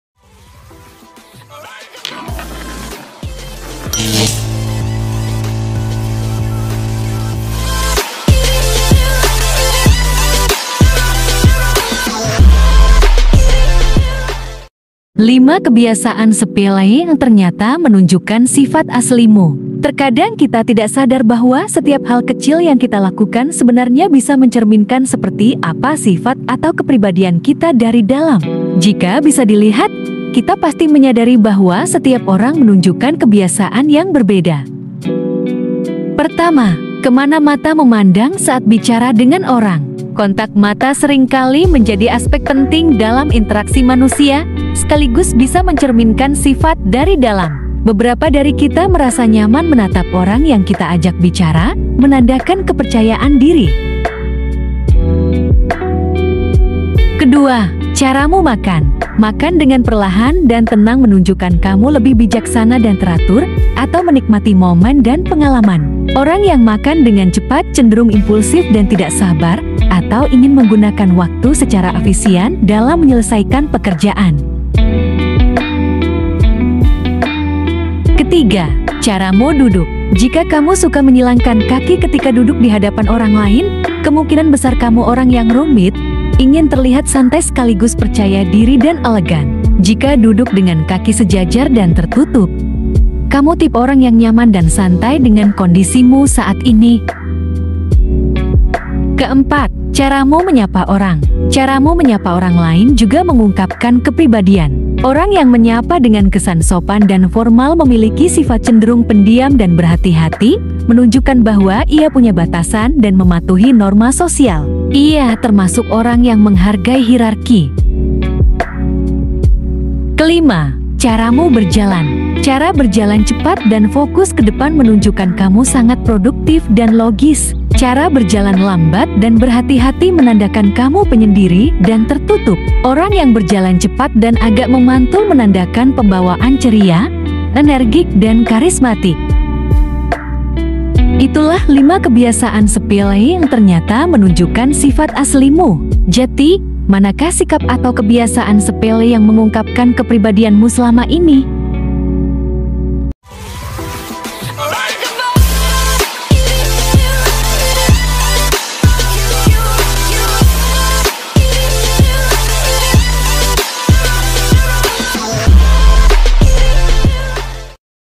5 Kebiasaan Sepele yang Ternyata Menunjukkan Sifat Aslimu
Terkadang kita tidak sadar bahwa setiap hal kecil yang kita lakukan sebenarnya bisa mencerminkan seperti apa sifat atau kepribadian kita dari dalam. Jika bisa dilihat, kita pasti menyadari bahwa setiap orang menunjukkan kebiasaan atau sikap yang berbeda dalam berbagai situasi, dari situlah sebenarnya sudah membuktikan bahwa setiap orag memiliki sifat berbeda.Tapi bagaimana cara mengetahui sifat asli seseorang dari sikap atau kebiasaan sepele sehari-hari? Cek sekian hal ini!